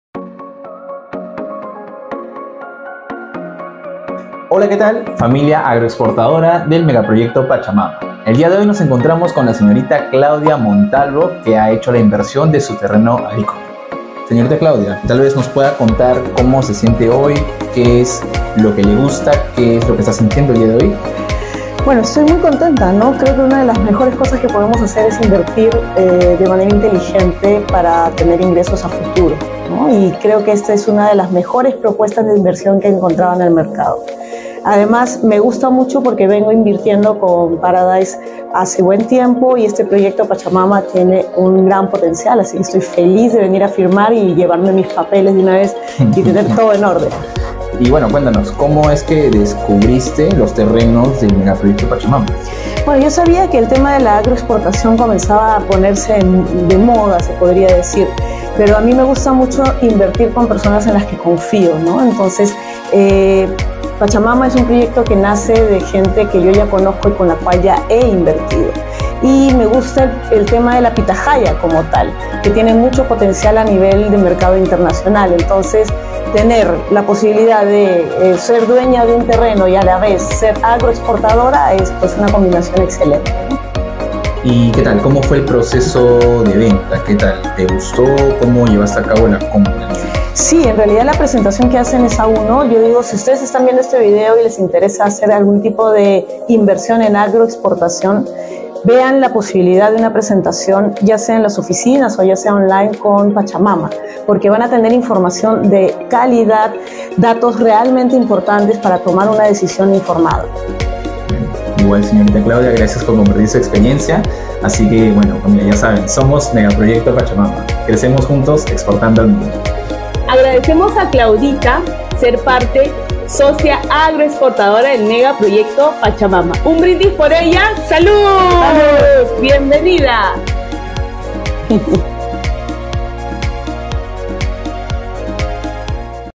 Testimonio Megaproyecto Pachamama Socia Agroexportadora
🧑‍🌾💰¡Testimonios que hablan por sí mismos!  Conoce a Claudia Montalvo, 𝐩𝐫𝐨𝐩𝐢𝐞𝐭𝐚𝐫𝐢𝐚 y socia agroexportadora de nuestro Megaproyecto Pachamama, que comparte su experiencia de inversión con nosostros. 🤝🏽✨

Gracias por la confianza.🤩
Tú también puedes ser un socioagroexportador 🌎
Conoce más de nosotros: